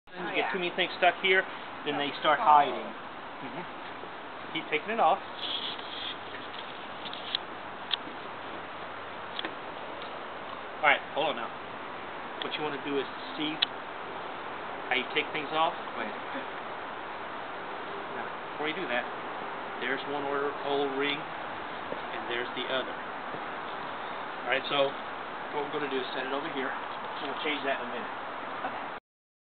Cassandra, Doing the oil change.
Cassandra doing th oil, and well this is her first time cause soon she will be driving.